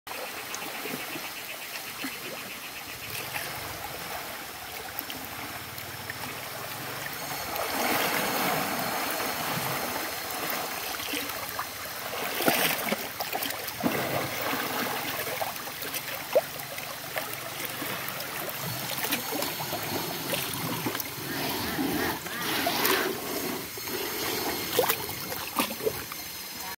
Itchy trunk
Bai-Ngern rolling around in the pool, but the fun was interrupted by an itch